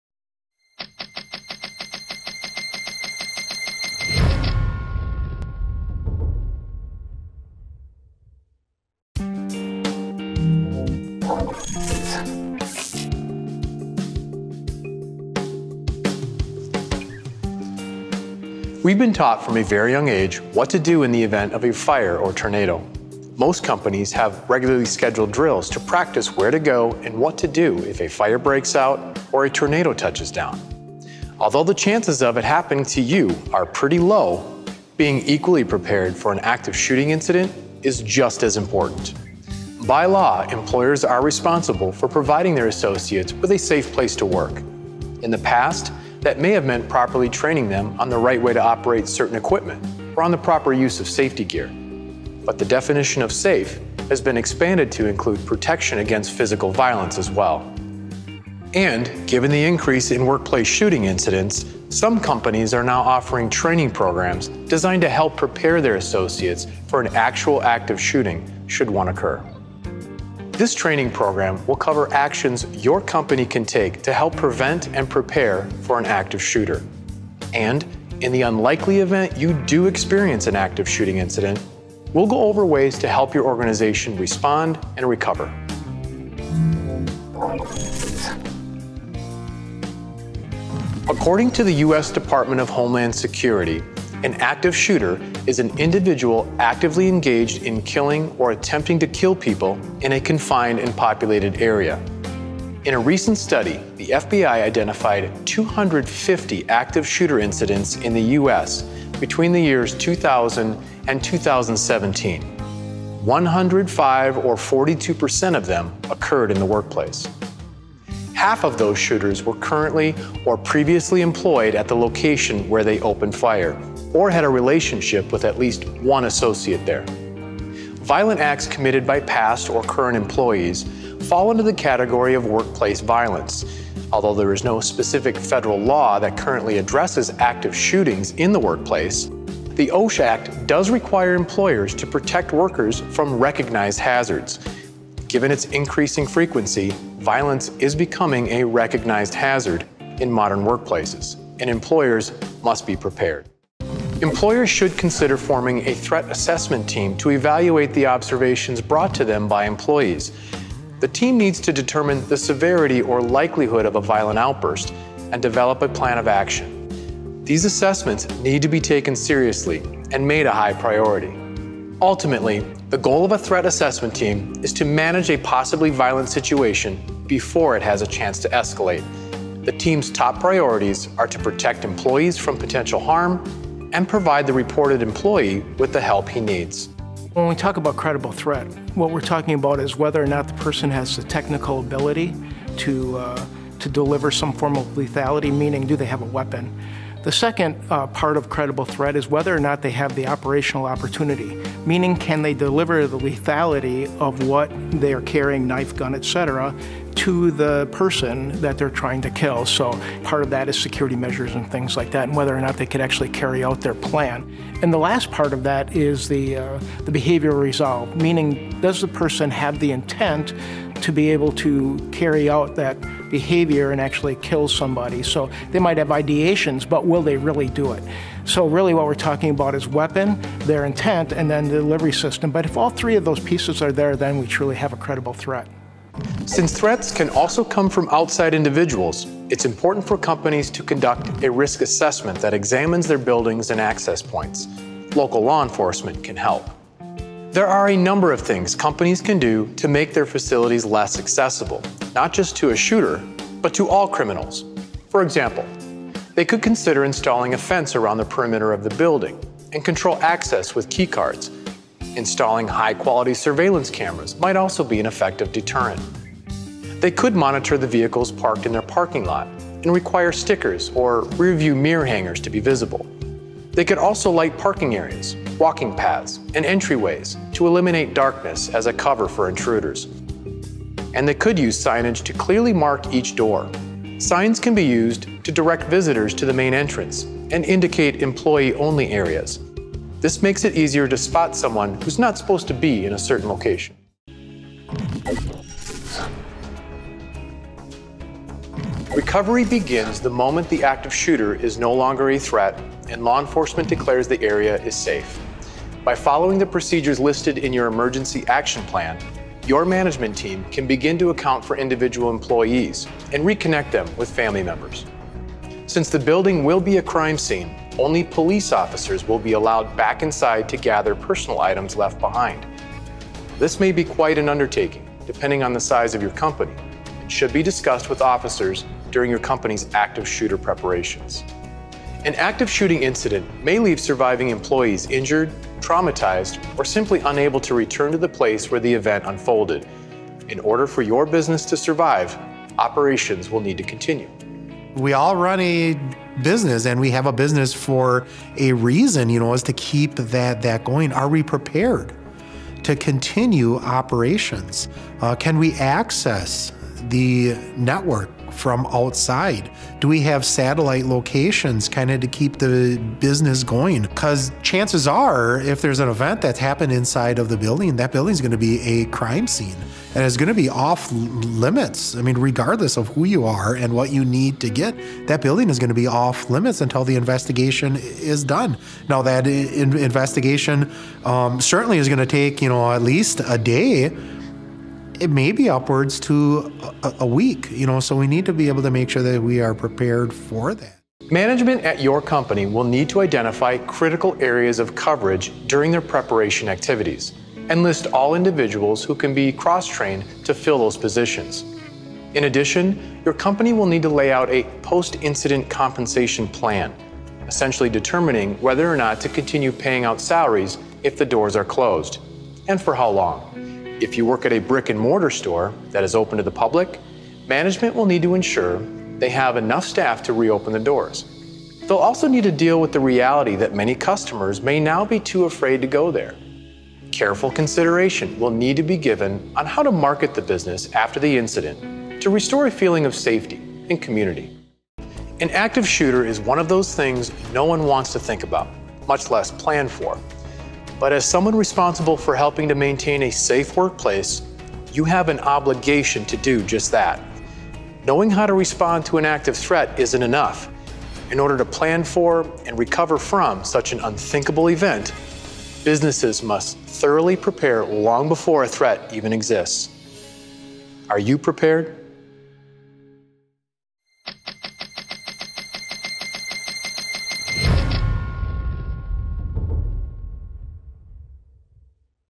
Active Shooter/Active Threat: Organizational Preparedness & Recovery
Helps members of management and Human Resources (HR) prepare employees for a potential threat incident.

Please keep in mind this is only a short preview of the full 22 minute training video.